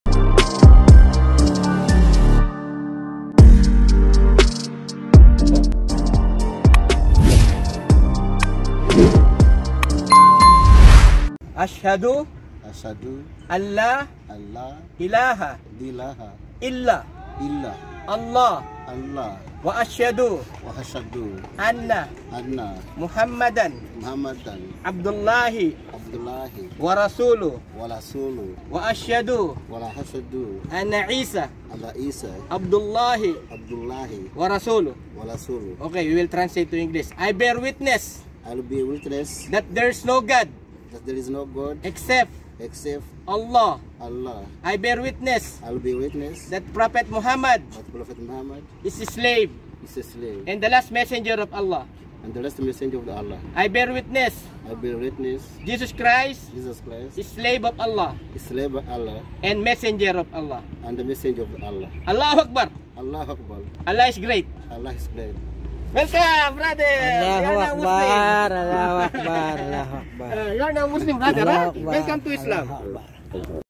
DAWAH SA BASKETBALL..DEC.17,2023 UGANDA CHRISTIAN EMBRACED ISLAM.
DAWAH BALIK ISLAM